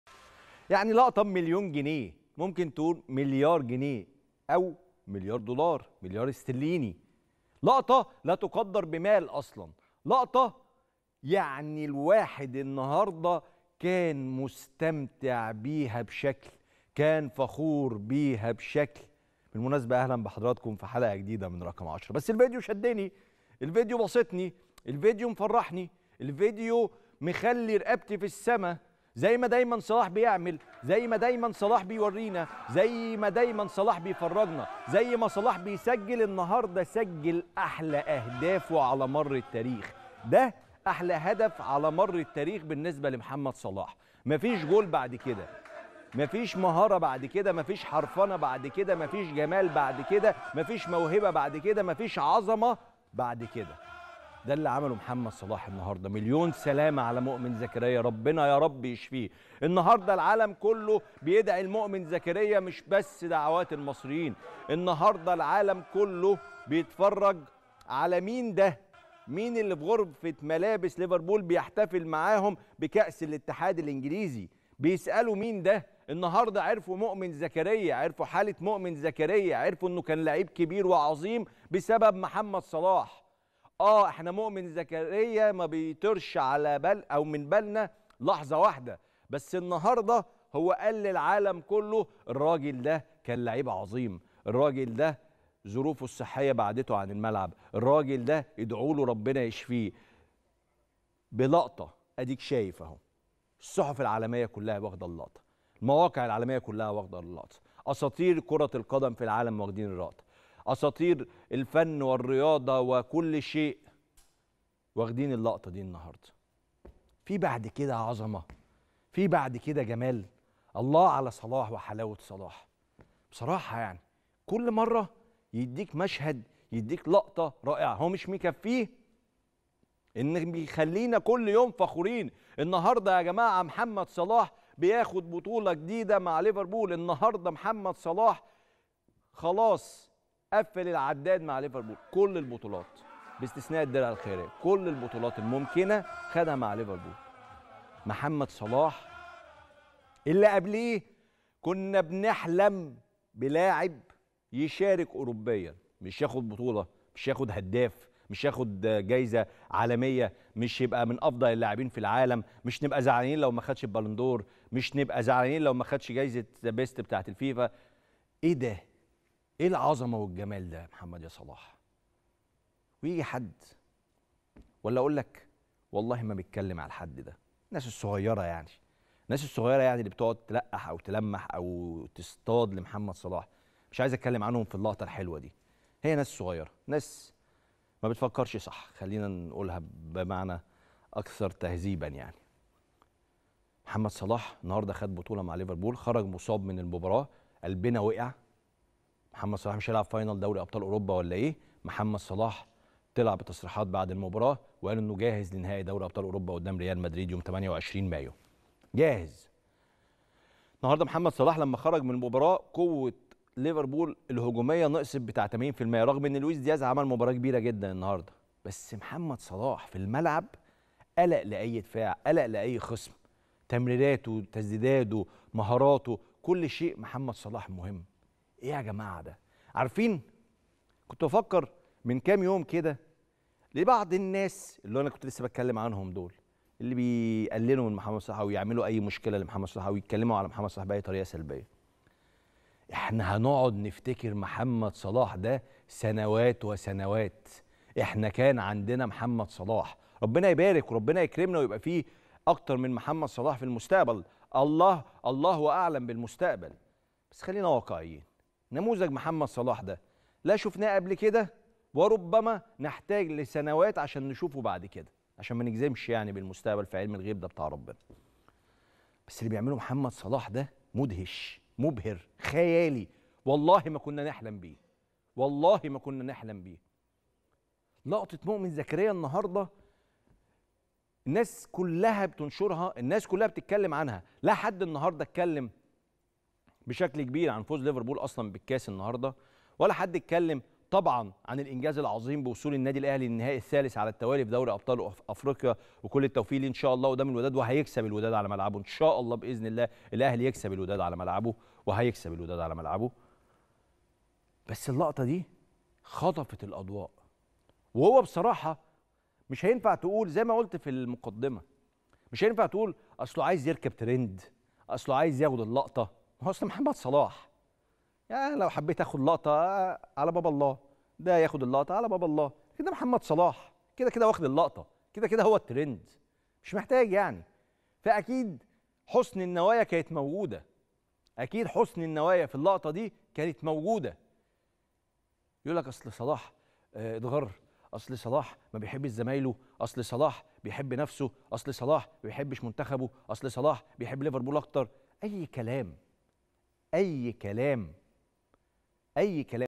كريم رمزي:مستحيل تقول ان صلاح بيدور على ترند أولقطة لان صلاح هو الترند نفسه وحسن النوايا كانت موجودة
رقم 10 | كريم رمزي: مستحيل تقول ان صلاح بيدور على ترند أو على اللقطة لان صلاح هو الترند نفسه وحسن النوايا كانت موجودة